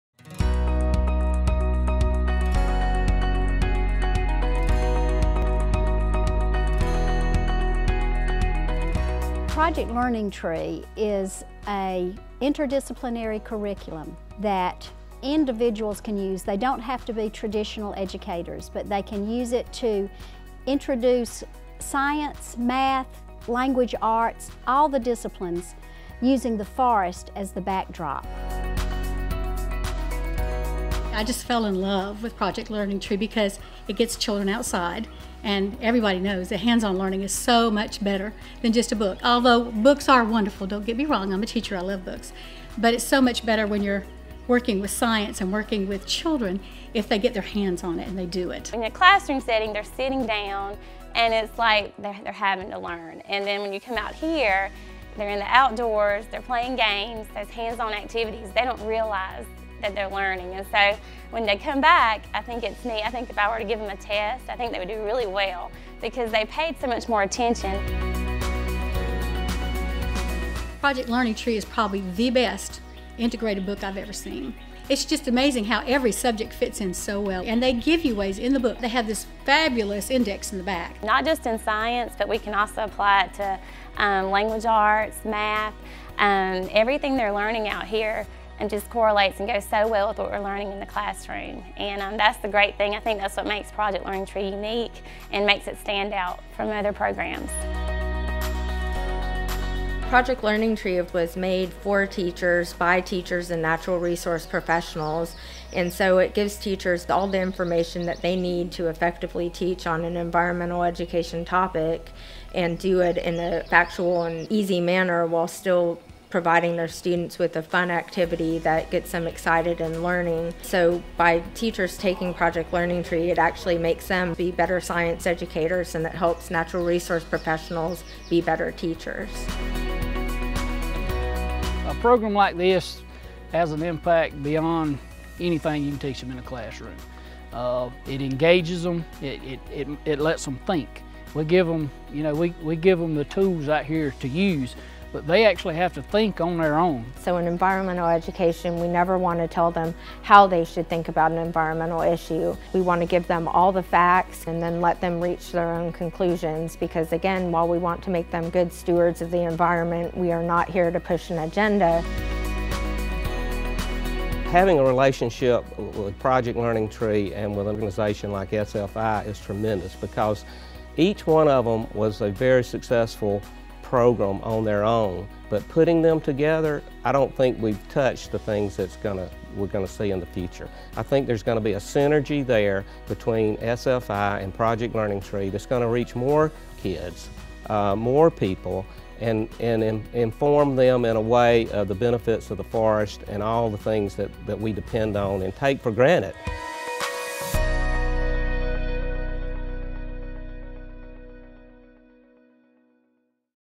PLT Uses Forests to Teach All Kinds of Subjects
PLT, an initiative of SFI, teaches youth about forests and the environment through hands-on, inter-disciplinary learning experiences that are correlated to classroom standards and get children outside.